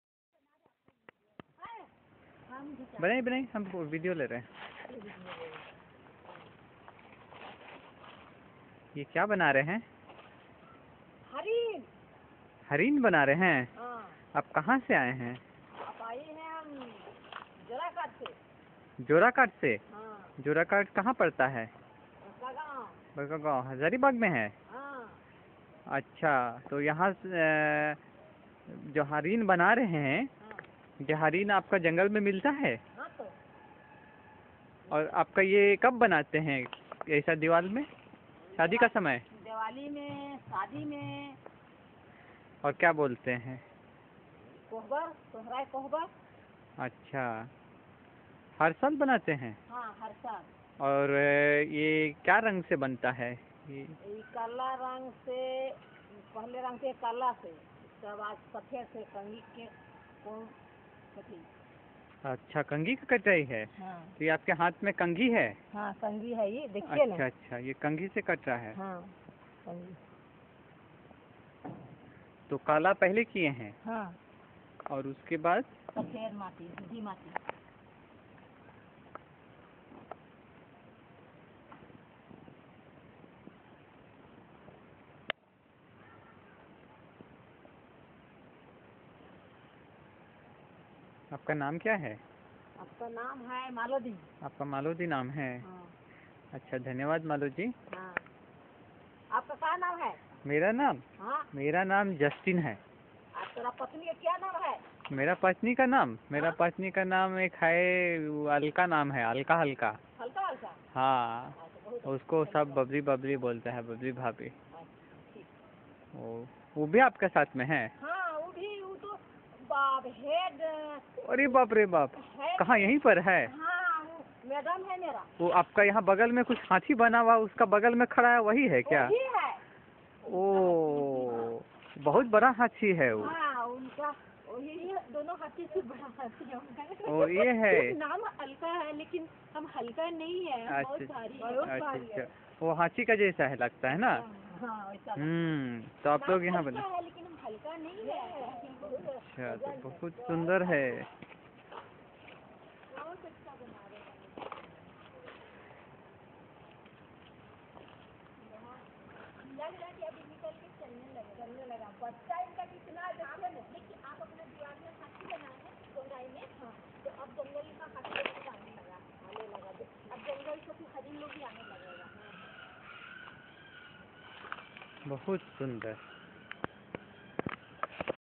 Sanskriti Khovar painting, Justin Imam, Nov. 2012
The Khovar (marriage art) is painted by scraping the white wet mud colour with broken pieces of combs or four fingers, to reveal the dark black inner layer to create beautiful designs and paintings, done by Tribal women in the Hazaribagh district of north Jharkhand in eastern India.